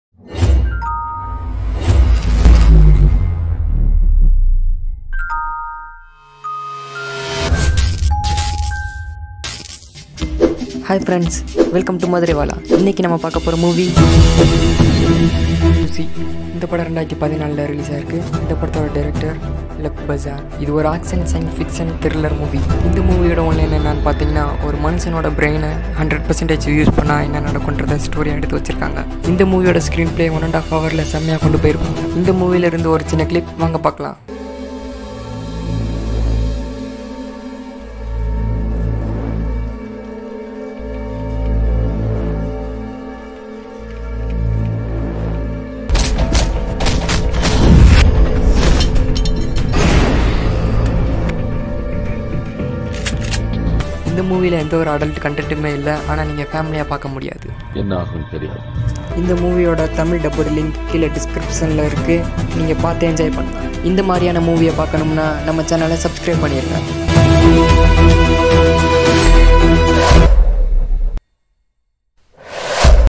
Lucy | Movie Review | Kutty Review Kutty Clip
Movie      : Lucy (2014)

Director      : Luc Besson

Starring      : Scarlett Johansson, Morgan Freeman

Genres      : Action, Sci-Fi, Thriller

Language   : Tamil

Rating      : 6.4/10